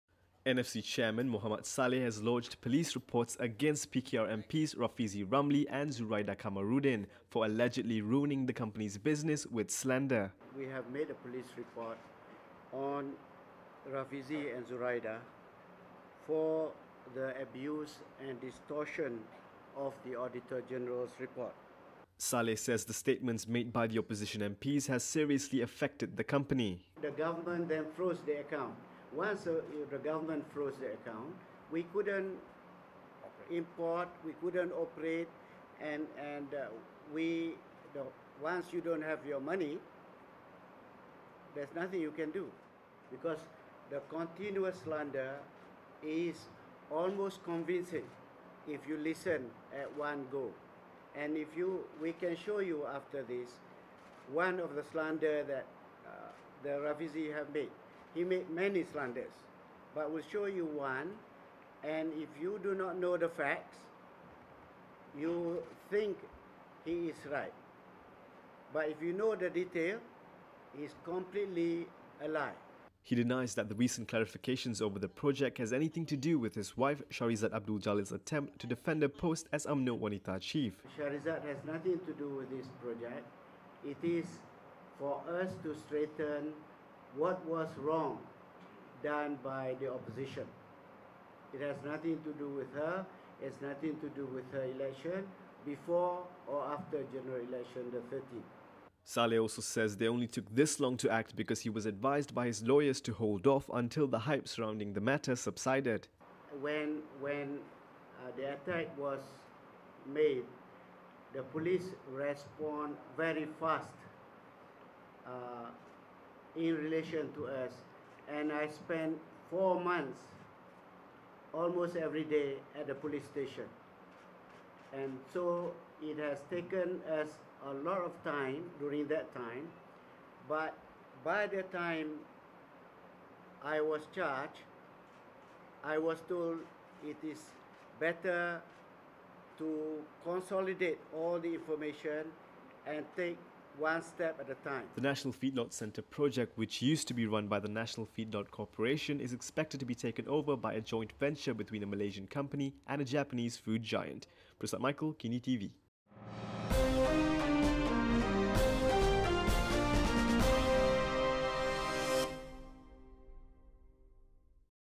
NFC lodges police reports against MPs for slander
NFC chairman Mohamad Salleh has lodged police reports against PKR MPs Rafizi Ramli and Zuraida Kamaruddin for allegedly ruining the company's business with slander.